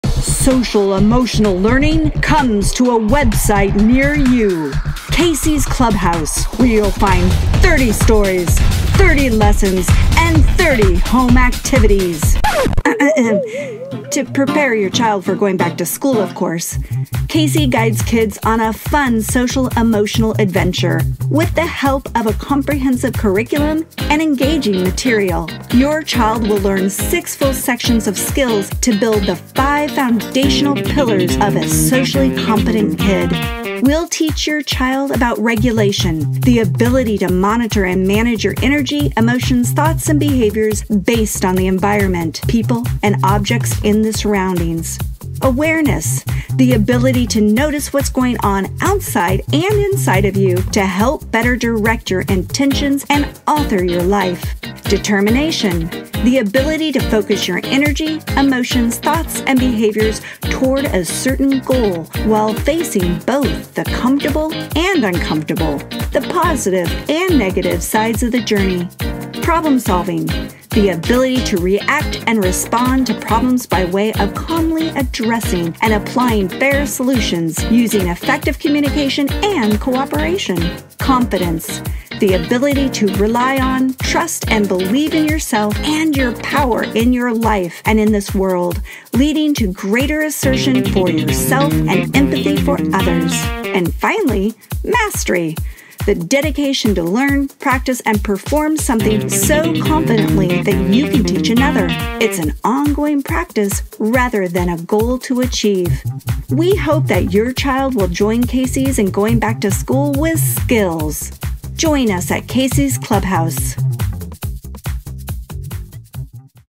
Casey's Clubhouse sends kids BACK TO SCHOOL with SOCIAL SKILLS!
Send your child back to school with skills alongside Casey and his friends! We offer a comprehensive 30-Video Curriculum that's completely animated to lead your child through an engaging and relatable social-emotional journey.